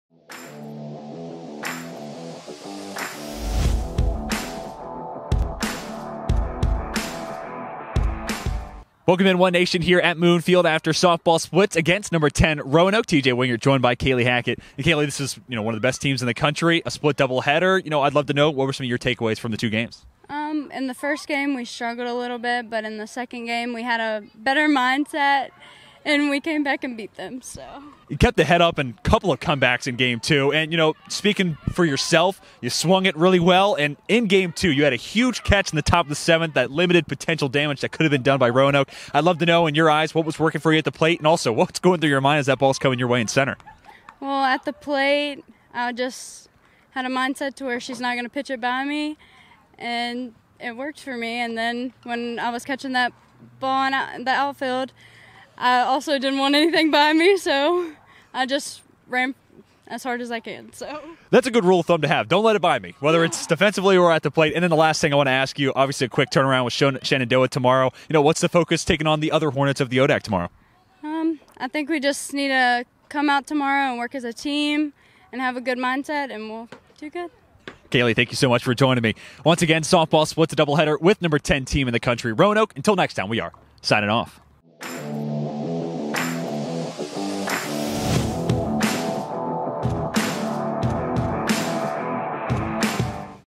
Postgame Interview: Kaleigh Hackett
Lynchburg softball plays another split doubleheader, this time #10 Roanoke. Sophomore Kaleigh Hackett had a big day at the plate and made a big catch in the top of the 7th of game 2 to preserve a Lynchburg win. Hear her thoughts postgame now!